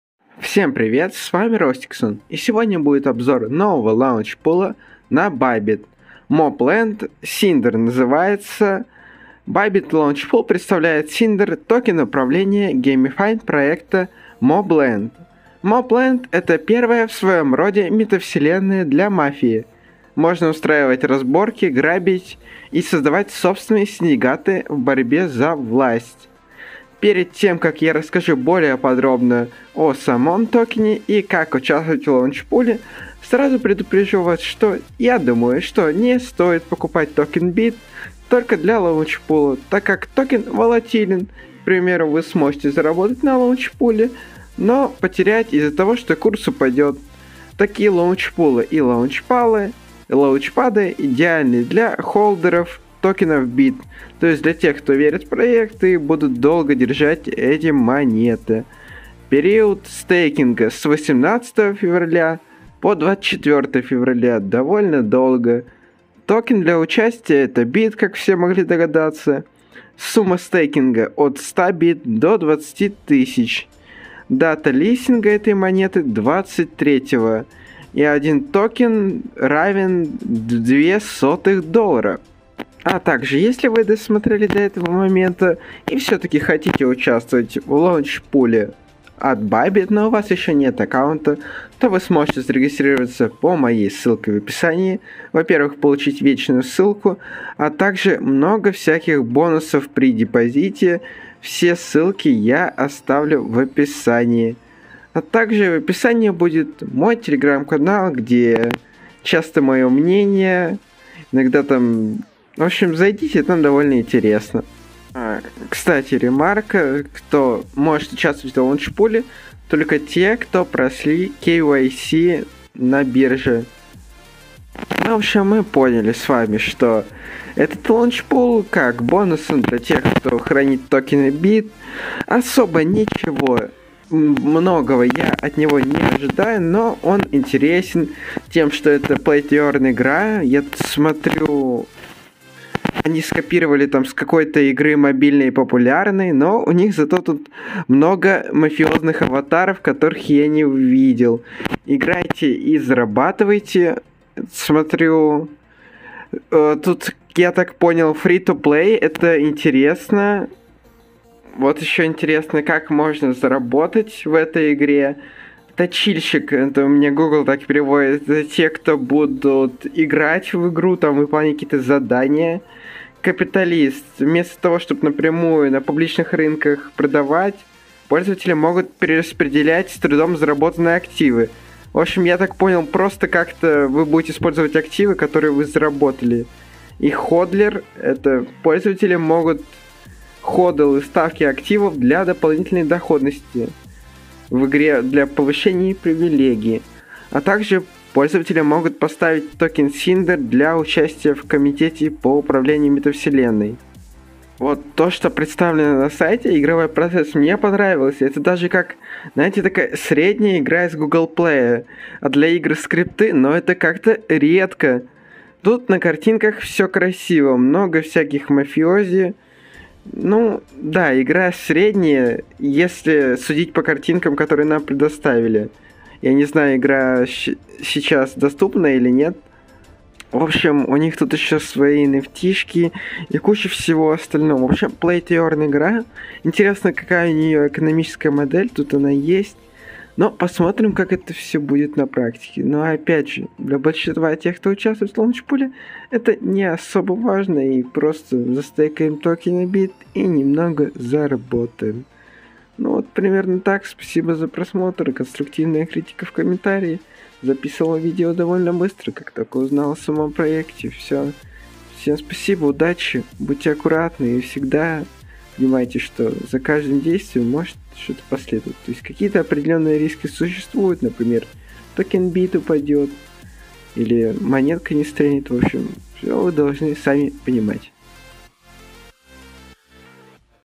✅Launchpool SYNR метавселенной MOB Land на Bybit!.Bybit Launchpool представляет $SYNR,Лаучпул Байбит
РЕГИСТРИРУЯСЬ ПО ССЫЛКЕ МОЯ ДЕВУШКА ПОЛУЧИТ БОНУС СПАСИБО
РЕФКА ДЕВУШКИ ОТ ДУШИ(процент с рефки ее)

MOBLAND это первая в своем роде Метавселенная для Мафии!
Можно устраивать разборки, грабить и создавать собственные синдикаты в борьбе за власть 😎

- Период стейкинга: 18.02.22, 10:00 utc - 24.02.22, 12:00 utc
- Токен для участия: BIT
- Сумма стейкинга: от 100 BIT до 20,000 BIT
- Дата листинга SYNR/USDT: 23.02.22, 12:00 UTC
- 1 токен SYNR = $0,02